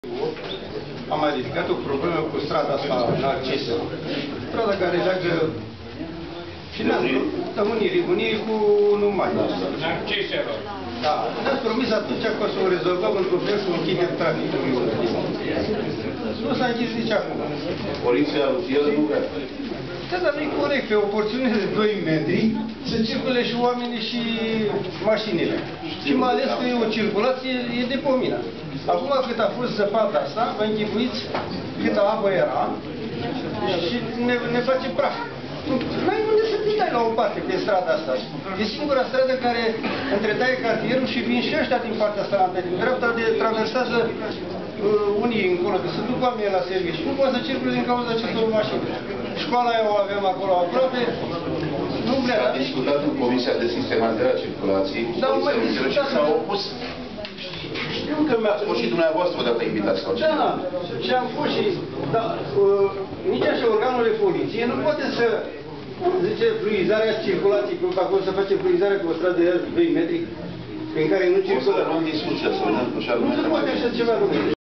Vocea cartierului organizat de Gazeta de Sud in cartierul 1 Mai din Craiova - partea a cincea